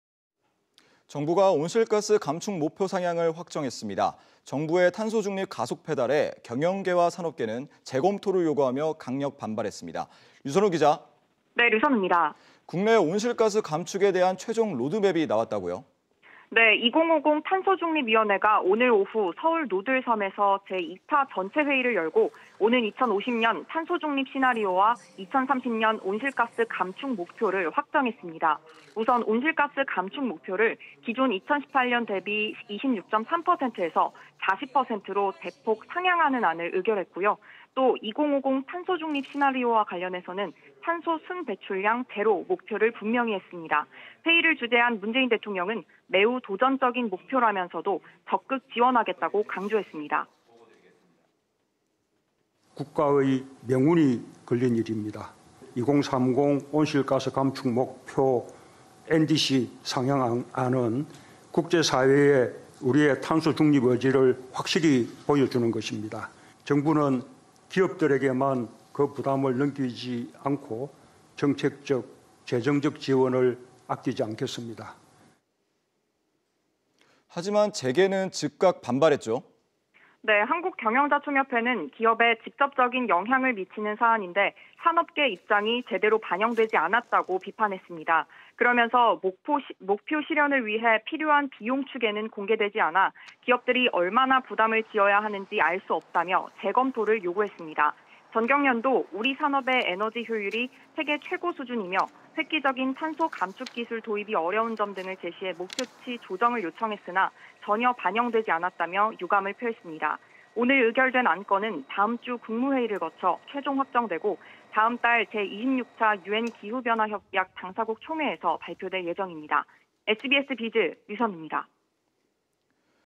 2030년 온실가스 40% 감축 확정…재계 “유감…재검토해야”
[앵커]

정부가 온실가스 감축 목표 상향을 확정했습니다.

정부의 탄소중립 가속 페달에 경영계와 산업계는 재검토를 요구하며 강력 반발했습니다.

류선우 기자, 국내 온실가스 감축에 대한 최종 로드맵이 나왔다고요?

[기자]

네, 2050 탄소중립위원회가 오늘(18일) 오후 서울 노들섬에서 제2차 전체회의를 열고, 오는 2050년 탄소중립 시나리오와 2030년 온실가스 감축 목표를 확정했습니다.